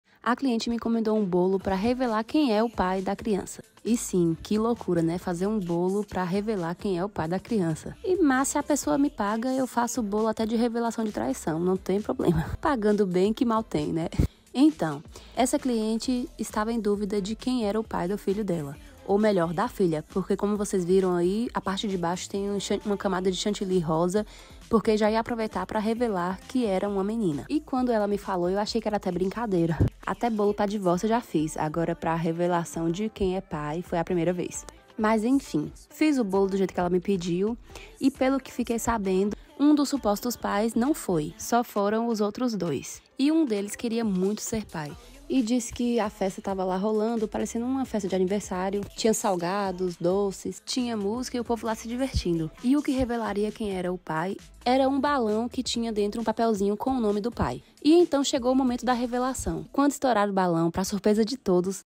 Chá revelação do pai do bebê? Confeiteira viraliza com pedido inusitado
Imagem: @confeitaria_da_leh / Reprodução